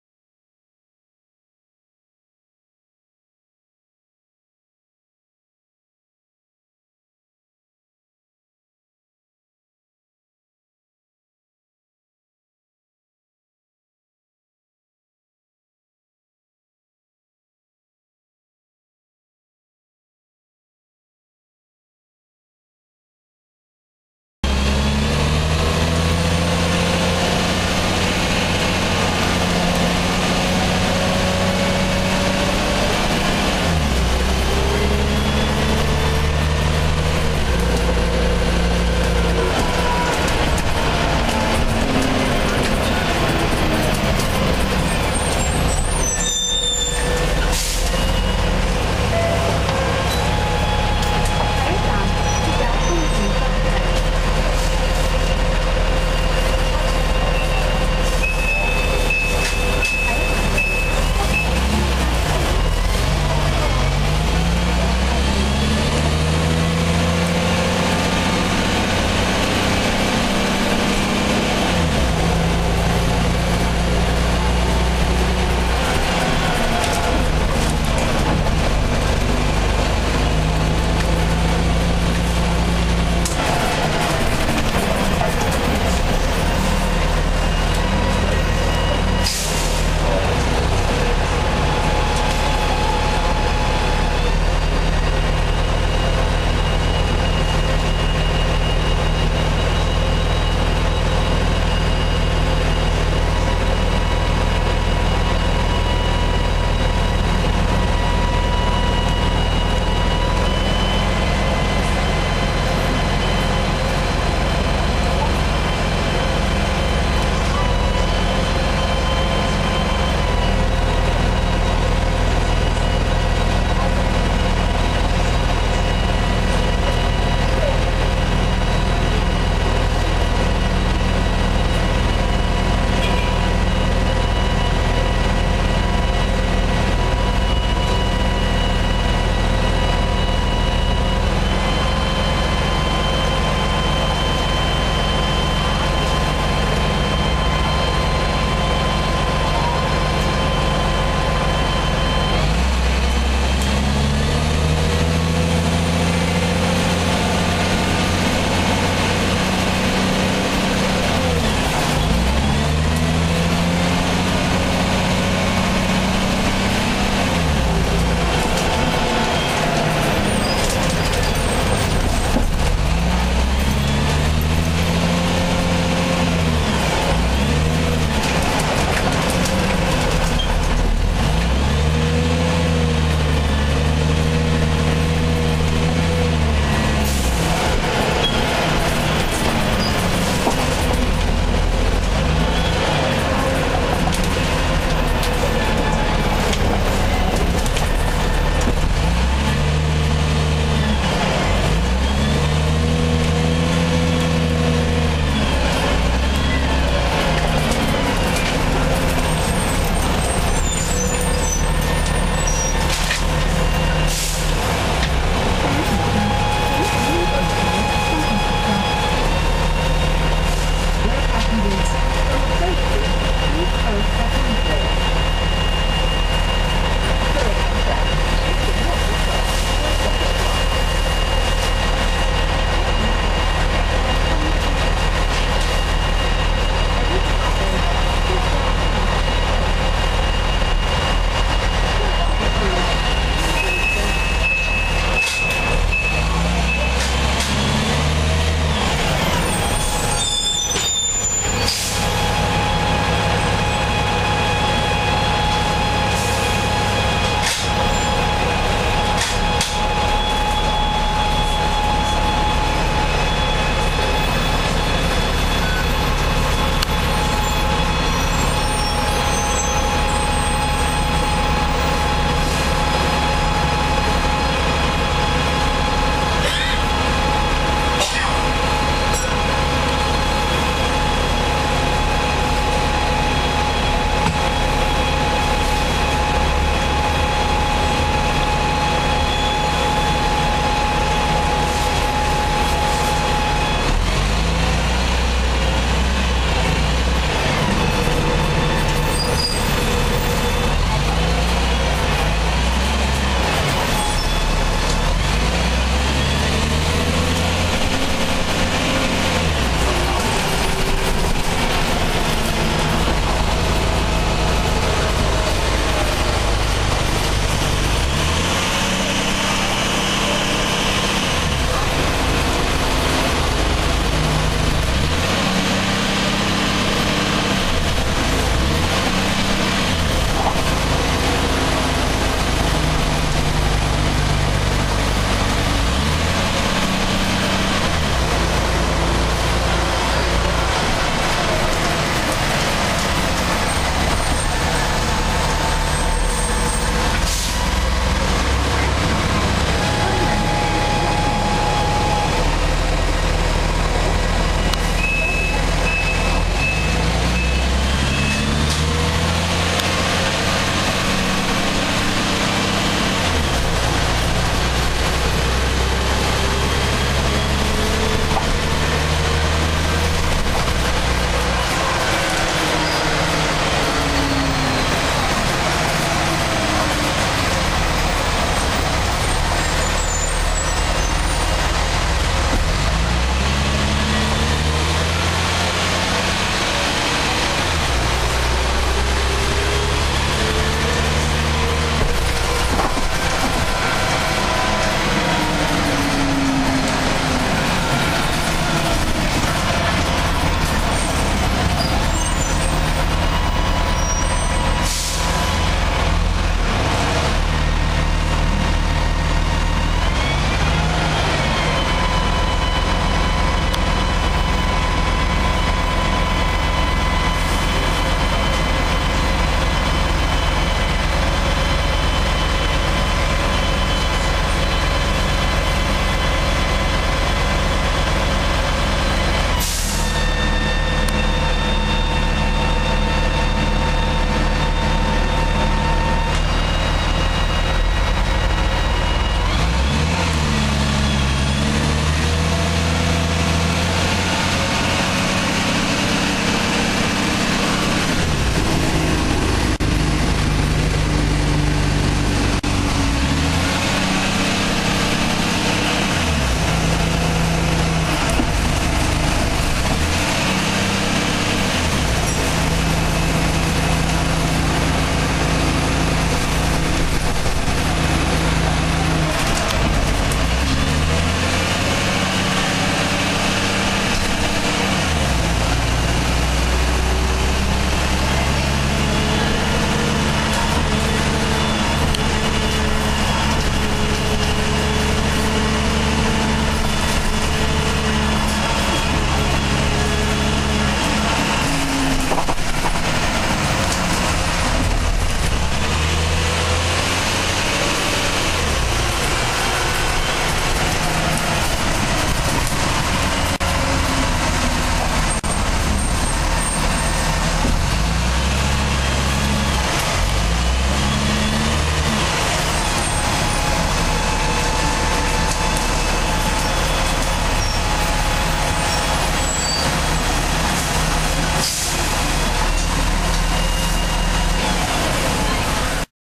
KMB Dennis Dragon AD105
A short trip on KMB's aircon Dennis Dragon AD105 (FK3292) through Kwai Chung on route to On Yam Estate terminus. These older aircon Dragons are fast approaching the end of their working lives, and AD105 will likely retire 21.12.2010 on its 18th birthday.

The Cummins LTA10-B282 engine and and Voith DIWA863 gearbox can be heard working hard on the climb up to On Yam in this November 2009 recording.